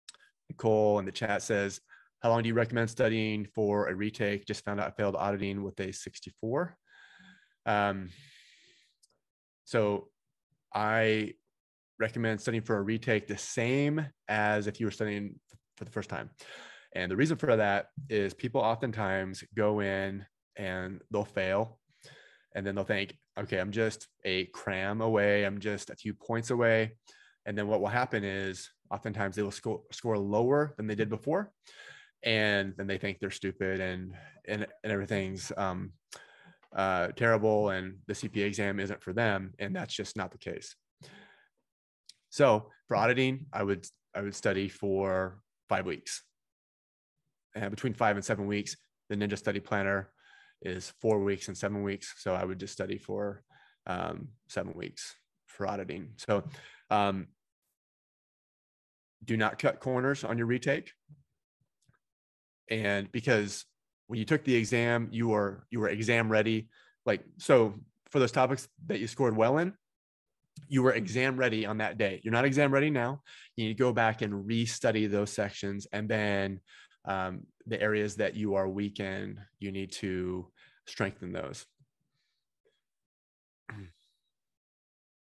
CPA Exam Retake Tips | How to Study AUD After Failing
Nicole asks "How long do you recommend studying for a CPA Exam retake? I failed Auditing with a 64."  Free CPA Notes ➤